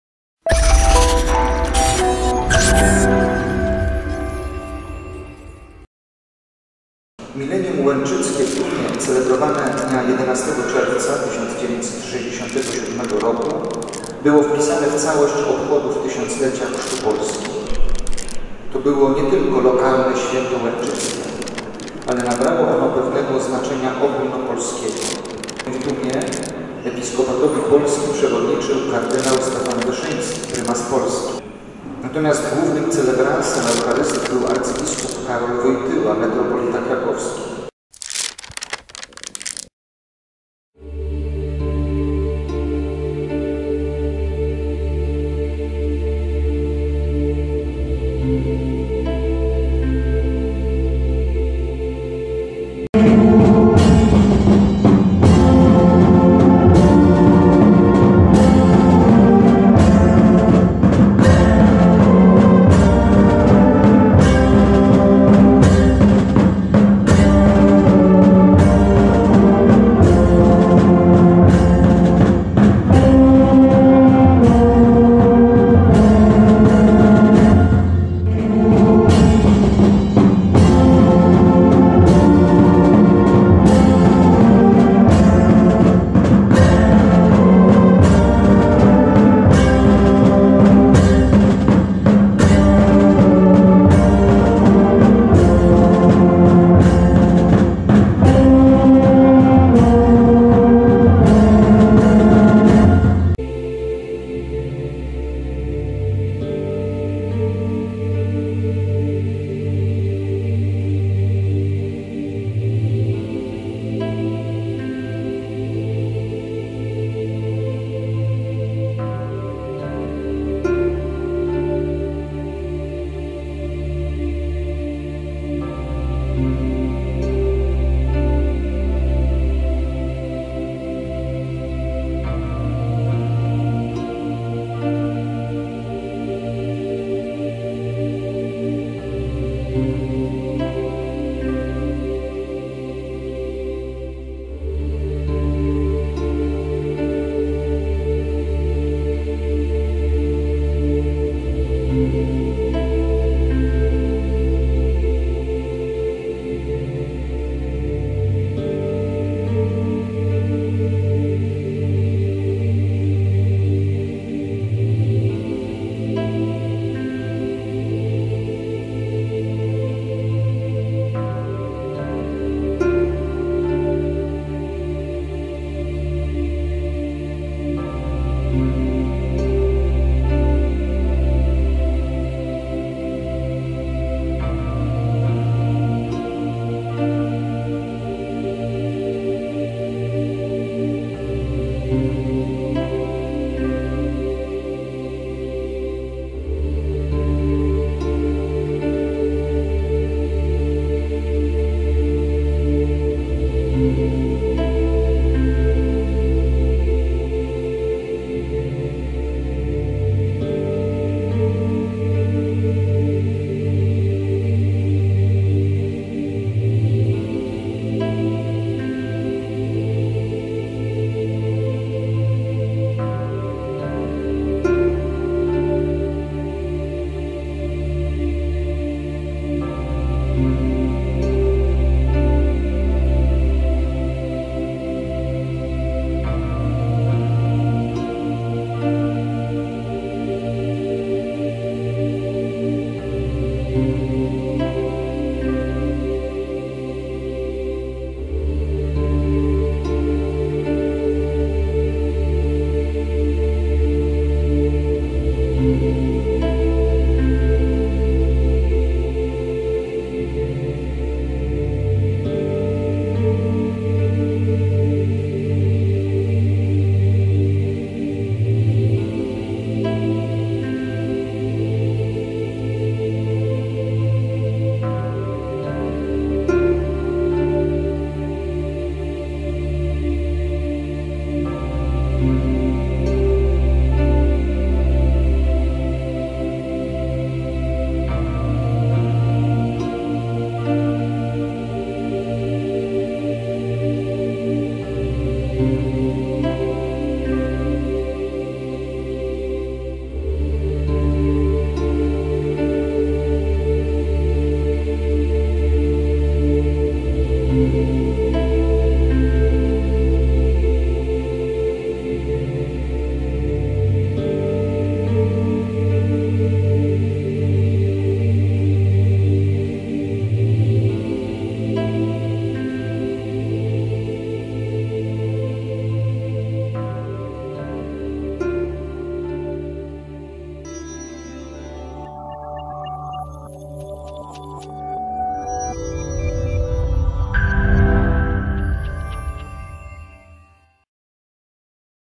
Obchody 50-lecia Milenium Chrztu Polski - Tum, Łęczyca 2017
Zapraszamy do obejrzenia relacji dokumentującej obchody 50-lecia Milenium Chrztu Polski, które odbyły się 25 czerwca 2017 r. w Archikolegiacie w Tumie oraz Królewskim Mieście Łęczycy. Wzięli w nich udział hierarchowie Kościoła: mszy św. przewodniczył kard. Stanisław Dziwisz, natomiast homilię wygłosił Prymas senior Abp Henryk Muszyński. Oprawę muzyczną prowadził chór Soli Deo z parafii pw. Św. Andrzeja Ap. w Łęczycy, a także Orkiestry dęte z Tumu, Góry św. Małgorzaty i Łęczycy.

Film zrealizowano na zlecenie Centralnego Łuku Turystycznego.